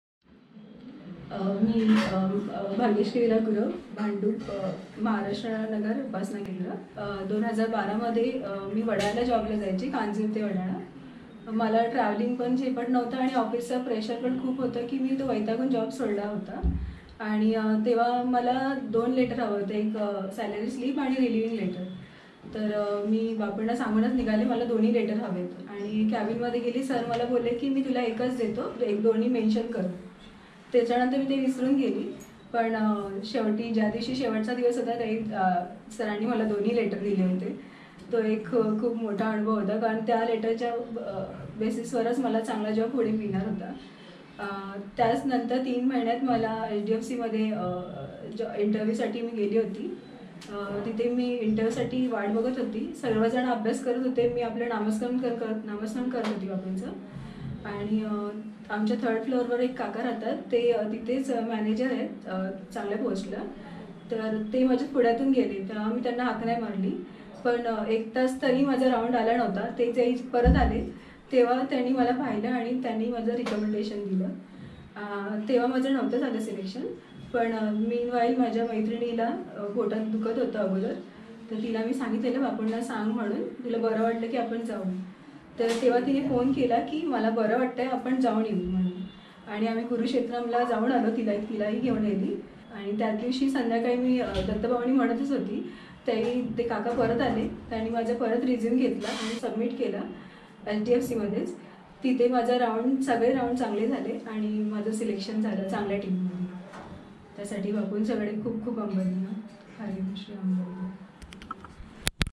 AniruddhaBapu - Experience in Marathi by Bhagyashree Gurav | Wish fulfilled | Job
Ms Bhagyashree Gurav from Bhandup narrates her experience of Sadguru Shree Aniruddha's grace.

Ms Gurav had to travel a long distance to reach her workplace daily. Hence, she resigned.

She got her relieving letter and salary slip without any hassle, based on which she would get a better opportunity.

She got an interview call. She felt anxious and was chanting her Sadguru's name.

Watch this video to know whether Sadguru Shree Aniruddha Bapu fulfilled the wish of this Shraddhavan child!